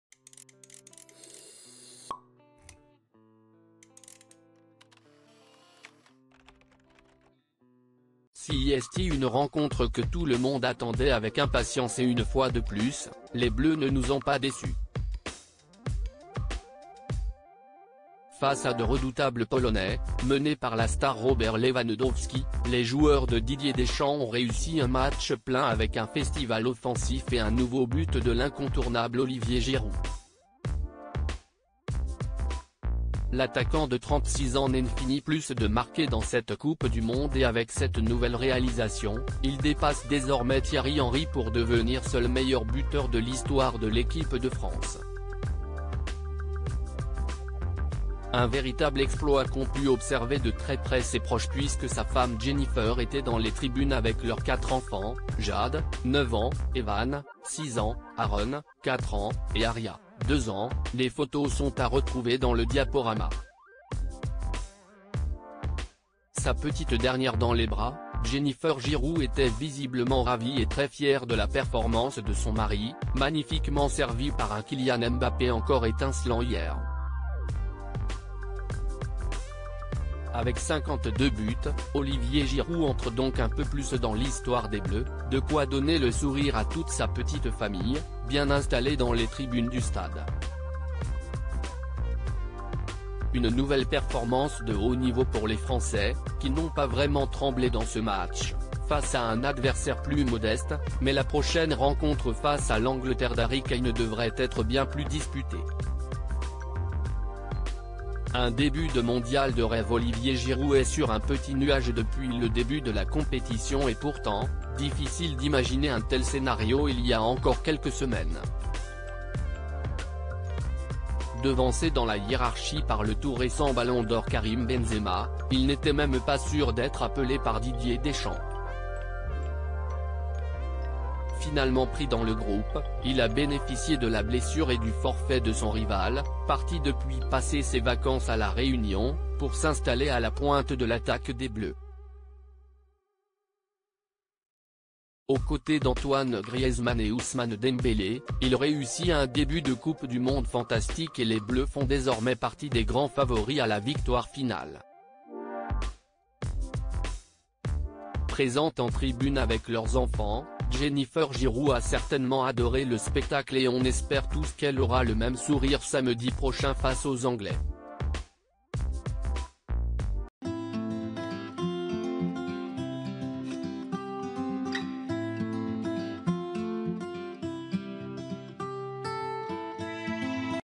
Coupe du monde : Olivier Giroud briseur de record, sa femme Jennifer et ses enfants à ses côtés pour
Coupe du monde : Olivier Giroud briseur de record, sa femme Jennifer et ses enfants à ses côtés pour ce moment historique !